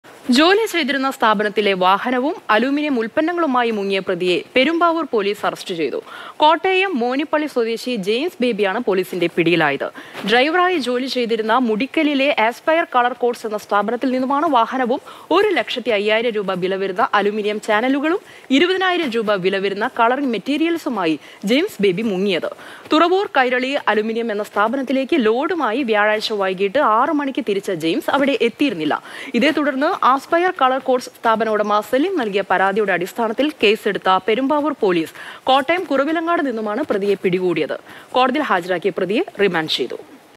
ജോലിക്ക് നിന്നിടത്ത് നിന്നും അലൂമിനിയം ഉൽപ്പന്നങ്ങൾ കടത്താൻ ശ്രമം; യുവാവ് പിടിയിൽ | Crime News
ജോലിക്ക് നിന്ന സ്ഥാപനത്തിൽ നിന്നും അലൂമിനിയം ഉൽപ്പന്നങ്ങളുമായി കടക്കാൻ ശ്രമിച്ച യുവാവ് പിടിയിൽ

Happy viewing!